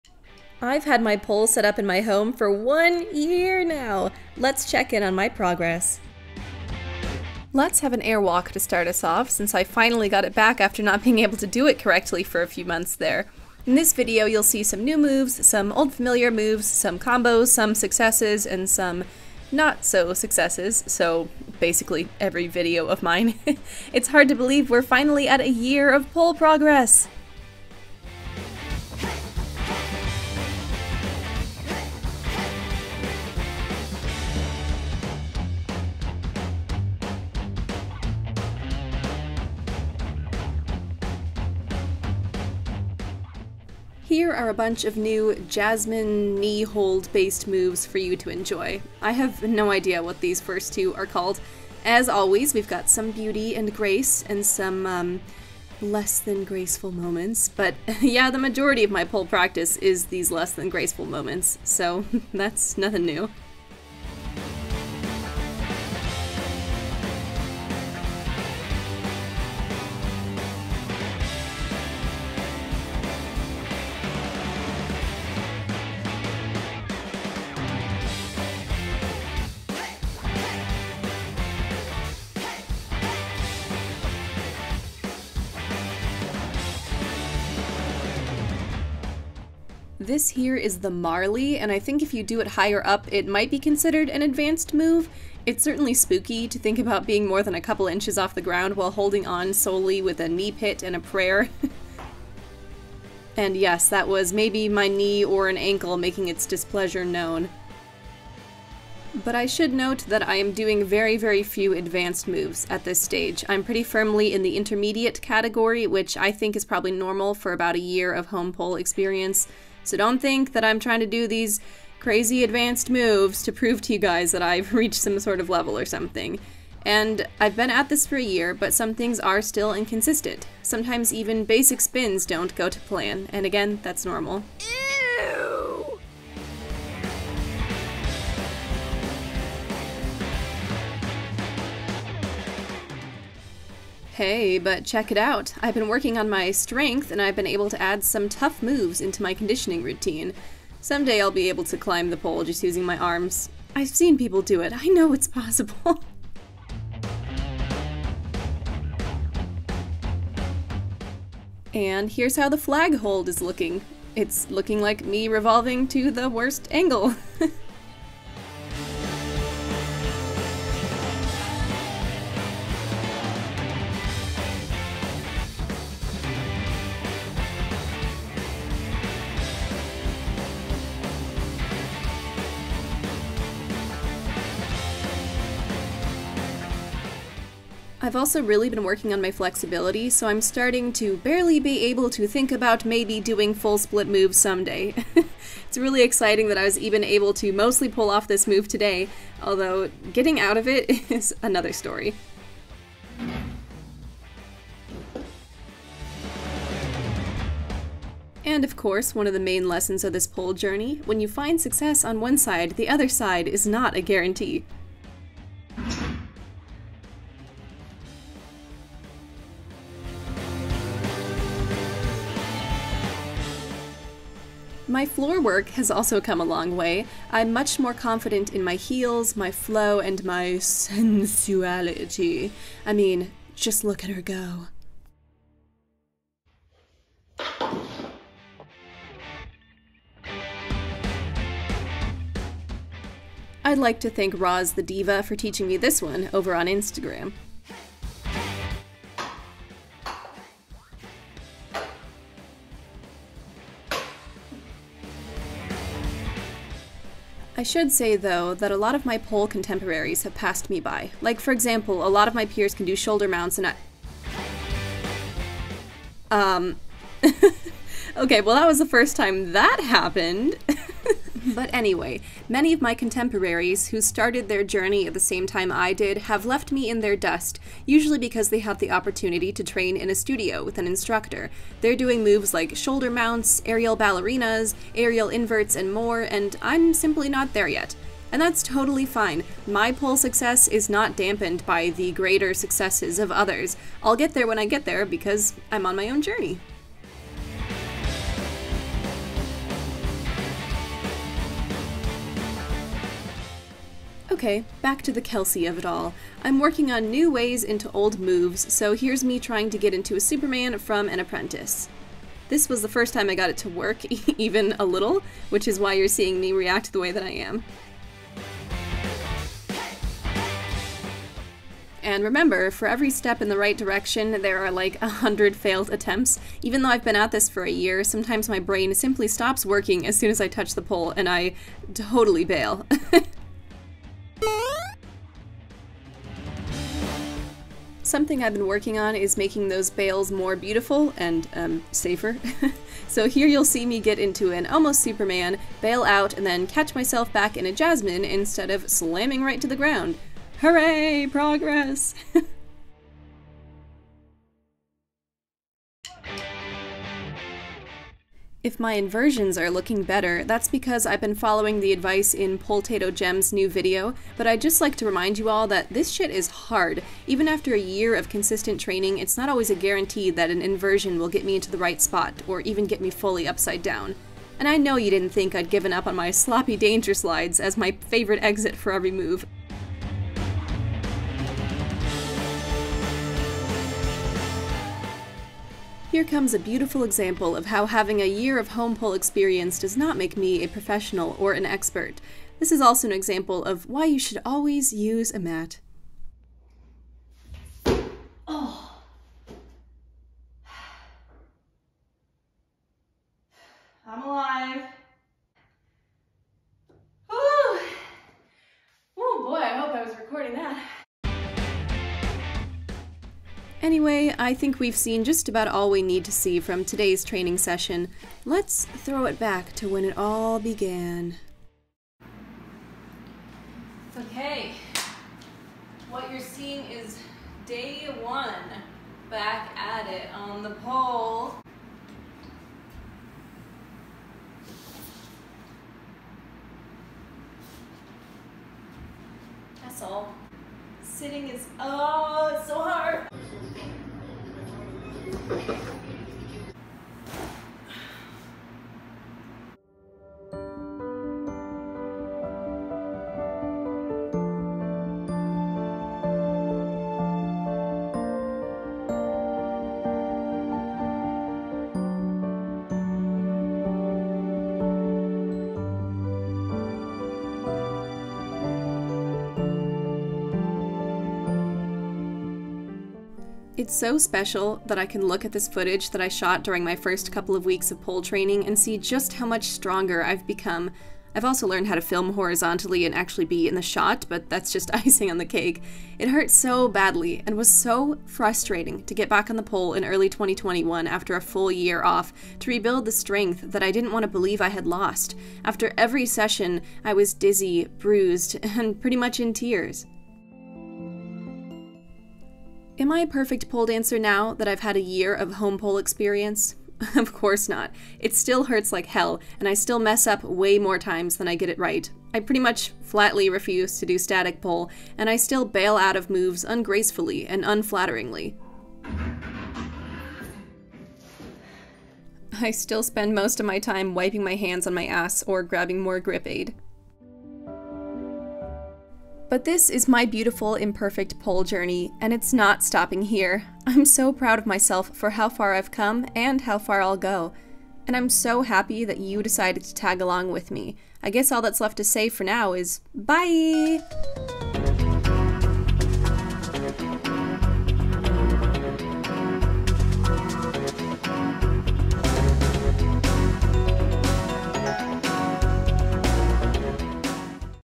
1 Year of Pole Progress!
This is my ONE YEAR home pole journey!! Can you believe we're finally here?
In this video, I'll be showing off some old familiar moves like the Airwalk, the Flag, Front Hook and Back Hook, and more, as well as variations on moves like Butterfly and Superman, plus we've also got brand-new moves like the Marley!

You'll hear me talk about how, despite my full year of experience on my home pole, some moves (like inversions) still don't happen every time, I still flop out onto the mat, and I still get bruised and upset. And unless I somehow magically become the perfect pole dancer, those things will continue to happen! It's all part of the beautiful journey!

I'd also like to note that a lot of my peers have advanced well beyond my level by now, because they train with instructors who can safely teach them new moves. That's totally fine! I'm going at my own pace, doing what feels safe on my own, and their success is not my failure.

References:
Roz the Diva on Instagram @rozthediva